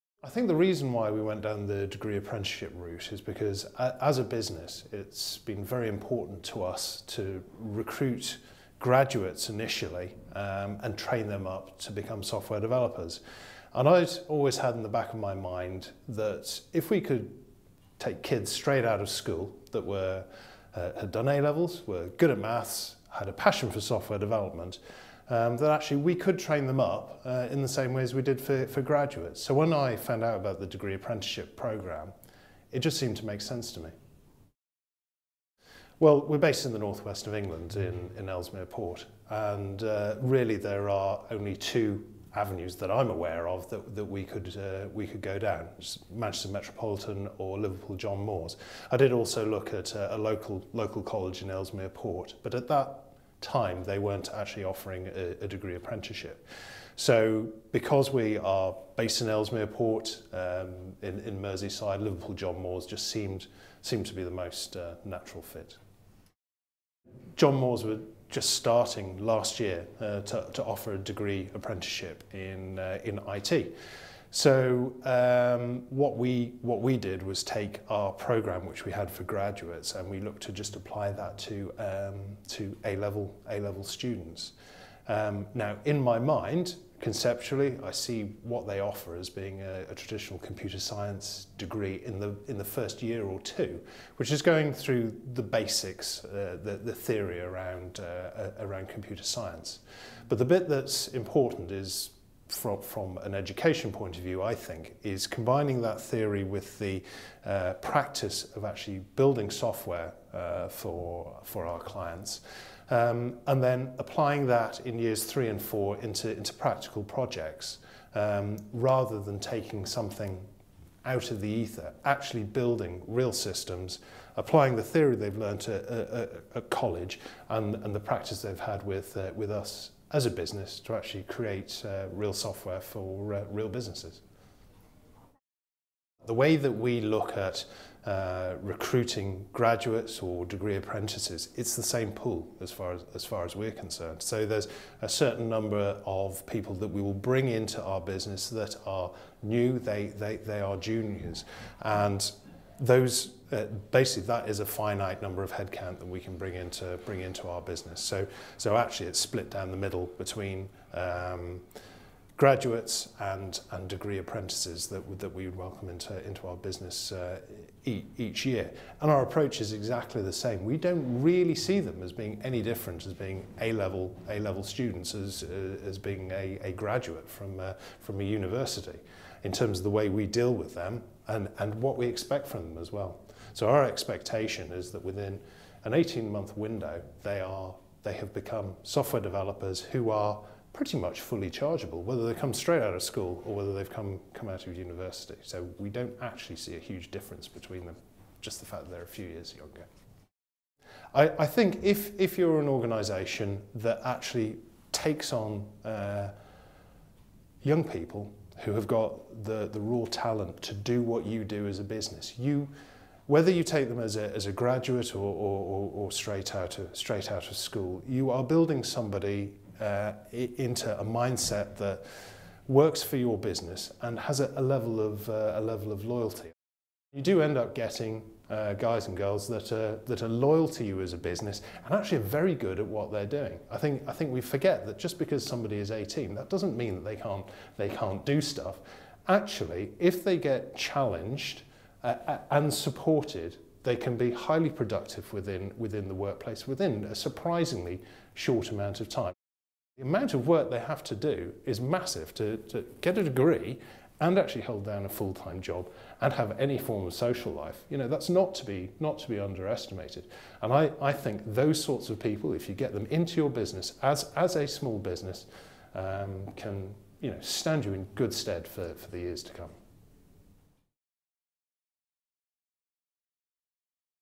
The Managing Director at 360-Systems explains the value of degree apprenticeships
David Jolliffe is managing director at 360-Systems, an employer of degree apprentices. Here he talks about the merit of employing degree apprentices.